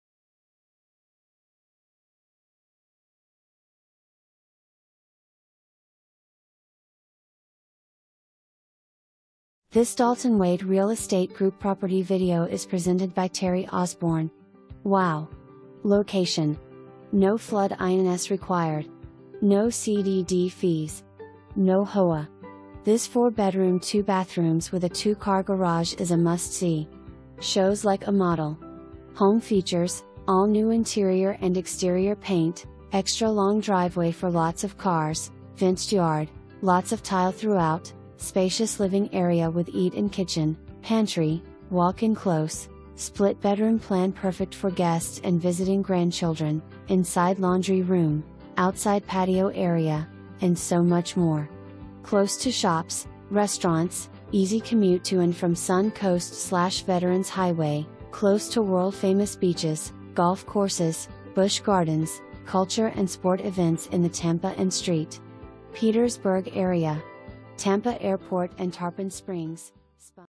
Dalton Wade Real Estate Group - 16384 Greenback Road
WOW!!! LOCATION!!! NO FLOOD INS REQUIRED! NO CDD FEES! NO HOA!!! This 4 bedroom two bathrooms with a two-car garage is a must see! Shows like a model! Home features: All new interior and exterior paint, extra-long driveway for lots of cars, fenced yard, lots of tile throughout, spacious living area with eat-in kitchen, pantry, walk in close, split bedroom plan perfect for guests and visiting grandchildren, inside laundry room, outside patio area, and so much more! Close to shops, restaurants, easy commute to and from Sun Coast/Veteran's Highway, close to world-famous beaches, golf courses, Busch Gardens, culture and sport events in the Tampa and St. Petersburg area. Tampa airport and Tarpon Springs, Sponge Docks. COME ENJOY FLORIDA'S LIFESTYLE AT ITS BEST!!!

2 full bath

Terri Osborn
Dalton Wade Real Estate Group